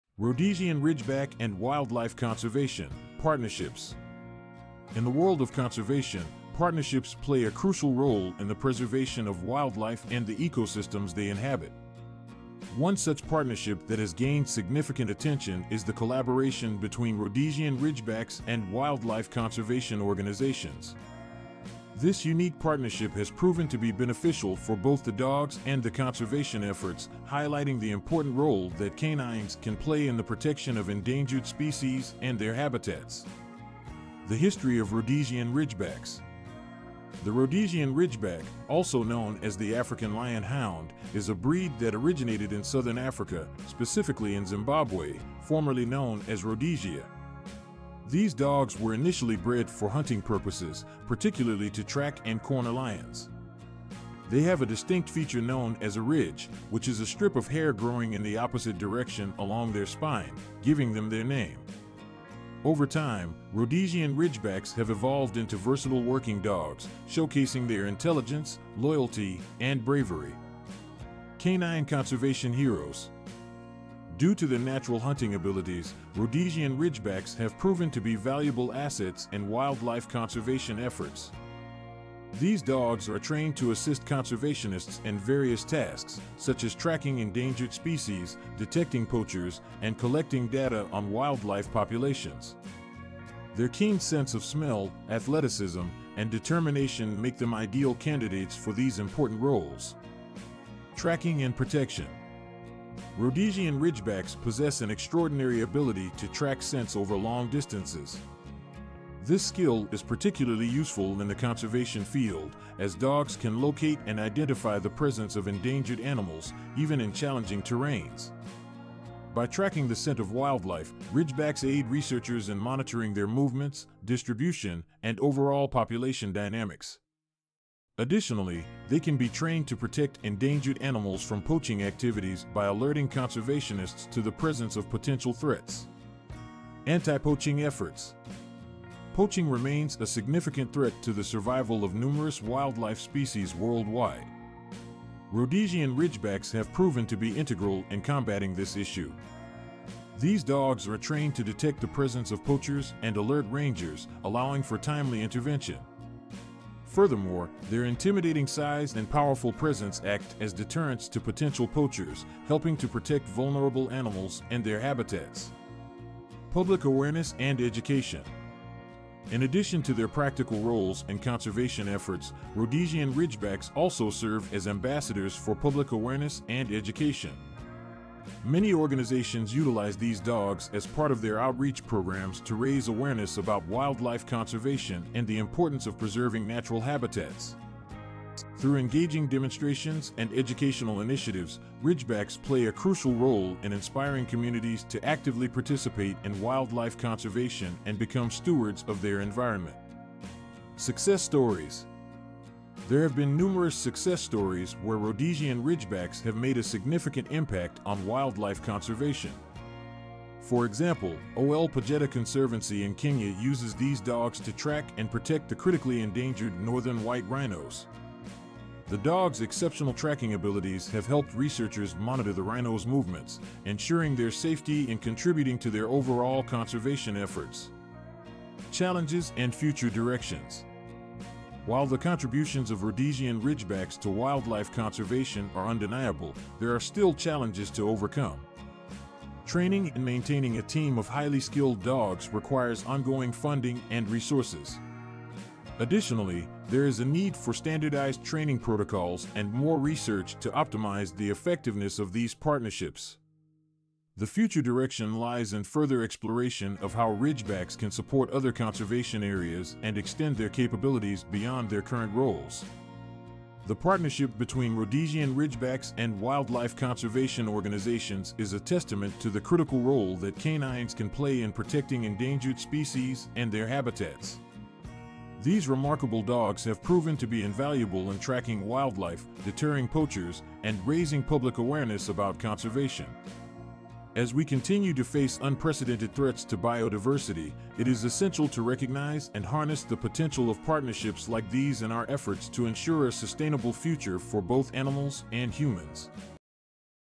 Rhodesian Ridgeback and Wildlife Conservation: Partnerships
In the world of conservation, partnerships play a crucial role in the preservation of wildlife and the ecosystems they inhabit
  One such partnership that has gained significant attention is the collaboration between Rhodesian Ridgebacks and wildlife conservation organizations
  This unique partnership has proven to be beneficial for both the dogs and the conservation efforts, highlighting the important role that canines can play in the protection of endangered species and their habitats
The History of Rhodesian Ridgebacks:
The Rhodesian Ridgeback, also known as the African Lion Hound, is a breed that originated in southern Africa, specifically in Zimbabwe (formerly known as Rhodesia)
  These dogs were initially bred for hunting purposes, particularly to track and corner lions
  They have a distinct feature known as a ridge, which is a strip of hair growing in the opposite direction along their spine, giving them their name
  Over time, Rhodesian Ridgebacks have evolved into versatile working dogs, showcasing their intelligence, loyalty, and bravery
Canine Conservation Heroes:
Due to their natural hunting abilities, Rhodesian Ridgebacks have proven to be valuable assets in wildlife conservation efforts
  These dogs are trained to assist conservationists in various tasks, such as tracking endangered species, detecting poachers, and collecting data on wildlife populations
  Their keen sense of smell, athleticism, and determination make them ideal candidates for these important roles
Tracking and Protection:
Rhodesian Ridgebacks possess an extraordinary ability to track scents over long distances
  This skill is particularly useful in the conservation field, as dogs can locate and identify the presence of endangered animals, even in challenging terrains
  By tracking the scent of wildlife, Ridgebacks aid researchers in monitoring their movements, distribution, and overall population dynamics
  Additionally, they can be trained to protect endangered animals from poaching activities by alerting conservationists to the presence of potential threats
Anti-Poaching Efforts:
Poaching remains a significant threat to the survival of numerous wildlife species worldwide